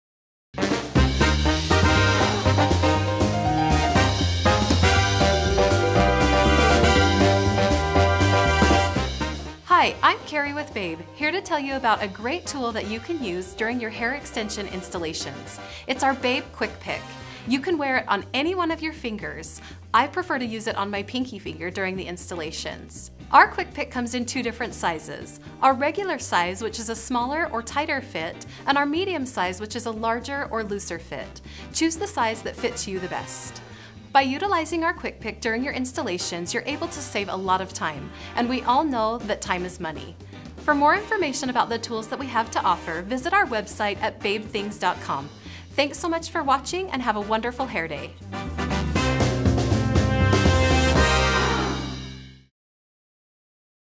Babe ‘Quick Pick’—Amazing Tool for Hair Extension Stylists
The Babe Quick Pick Hair Parter is the perfect tool for parting and sectioning hair. It replaces the rattail comb and saves tons of time during the installation process. The Babe Quick Pick comes in two sizes for a perfect fit. Find yours now: